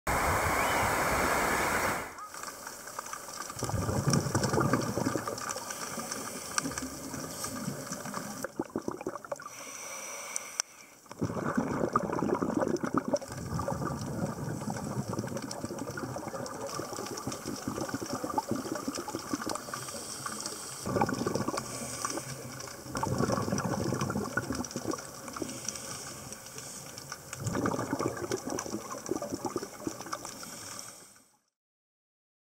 命育む分布北限域のサンゴ群落 静岡・内浦湾のエダミドリイシ
駿河湾最奥部、静岡県沼津市の内浦湾は造礁性サンゴの分布北限域に含まれる。厳しい環境を生き抜く造礁性のサンゴは、魚の餌場となるほか幼魚やエビ・カニなどの隠れ家にもなり、命を育む。